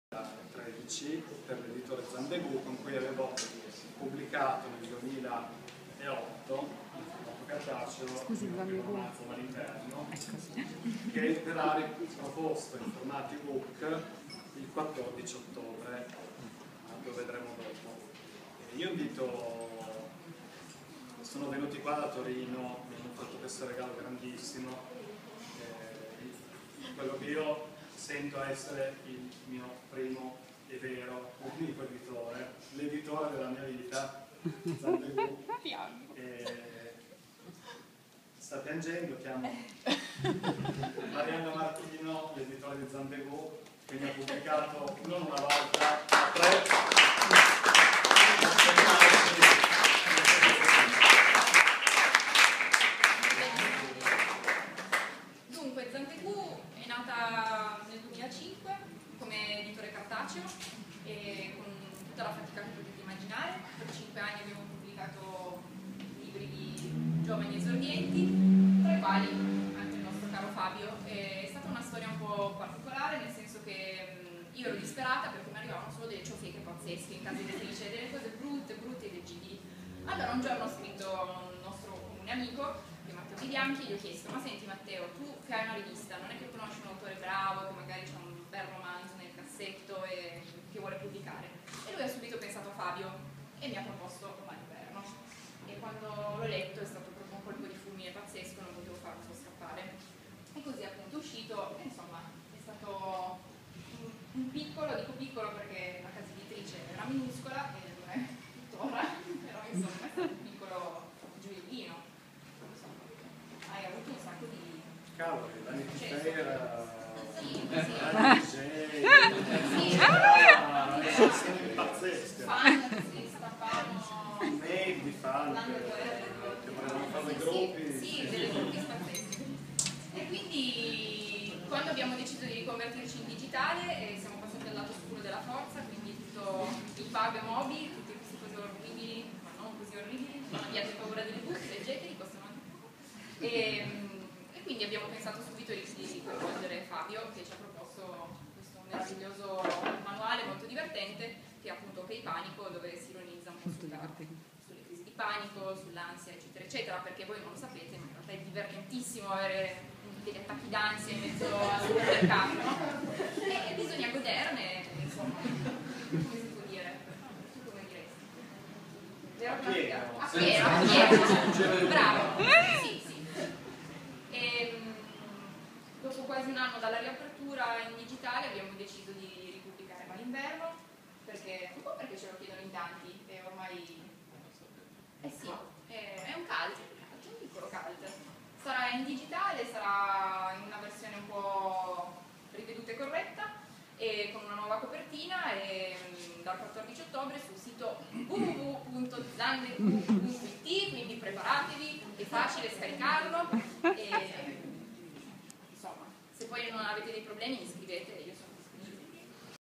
07 Marianna Martino di Zandegù
Musica, vino e letture tratte dai libri di Fabio Lubrano - Wineria 19 settembre 2013 - Marianna Martino racconta la sua casa editrice Zandegù